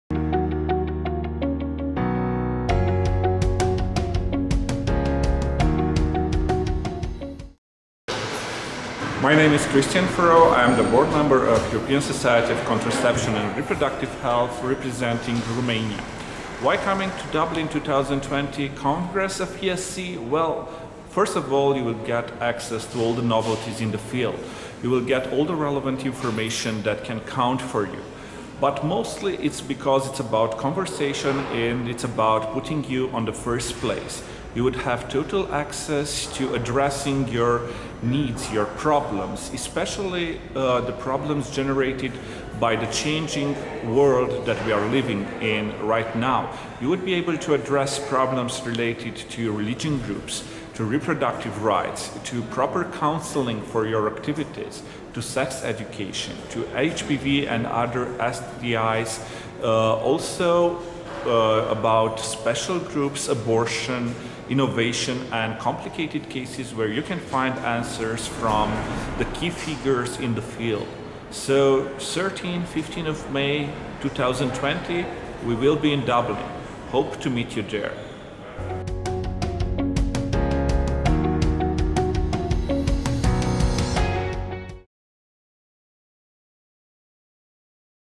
Cristian Furau, ESC Board Member Romania -Welcome to Dublin for the 16th ESC Congress, 13-15 May '20
Why come to Dublin? Access to all the novelties in the field but mostly it is about conversation and putting you in the first place. Total access to addressing your needs and problems especially the problems generated by the changing world that we are living in right now: religion groups, reproductive rights, proper counselling, sex education, HPV, STIs, special groups, abortion, complicated cases. You will find answers from the key figures in the field. 13-15 May, we will be in Dublin. Hope to meet you there.